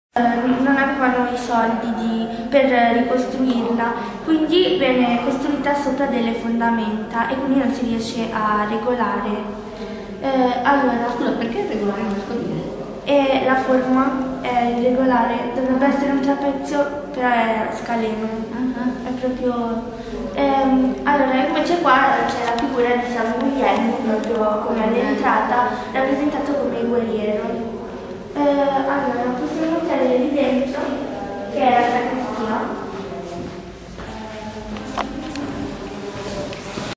Cappella della Chiesa di San Guglielmo, Castellazzo di Bollate
Cappella della  Chiesa di San Guglielmo, Castellazzo di Bollate, visitata durante Insieme per le Groane 2012